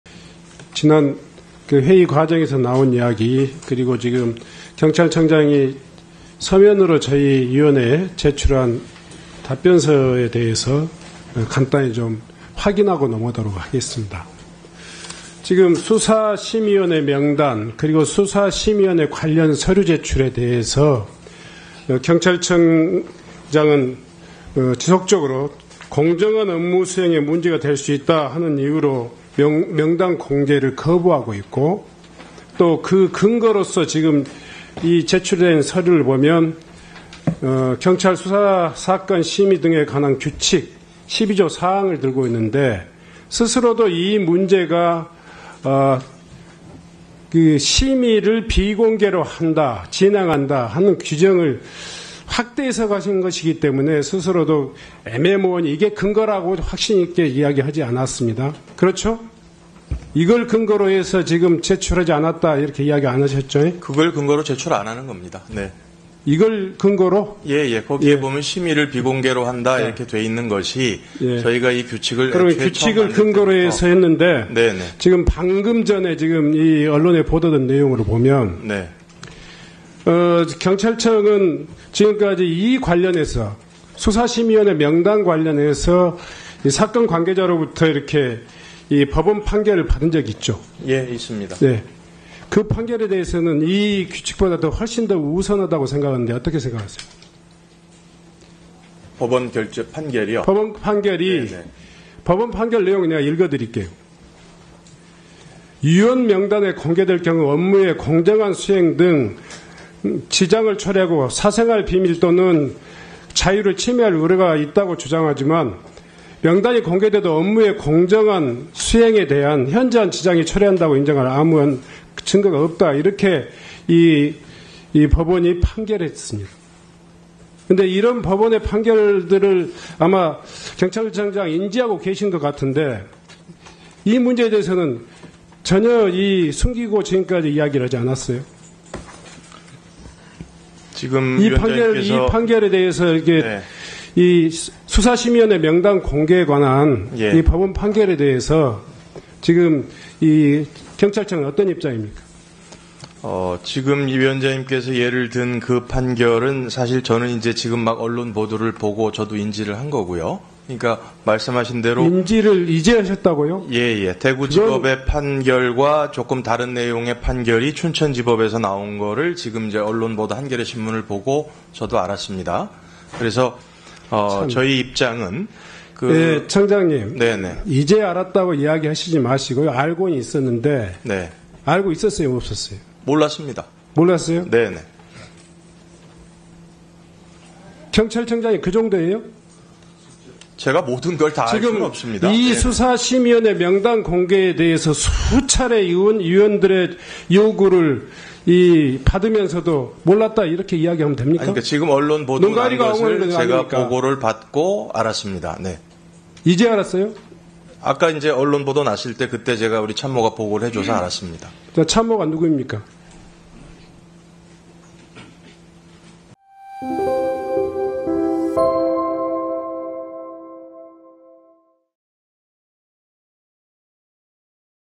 임성근 무혐의 준 수심위 명단 비공개 주장하다...공개 OK 법원 판결에 경찰청장 급당황
2024년 7월 11일 국회에서
행정안전위원회 전체회의가 진행되었다.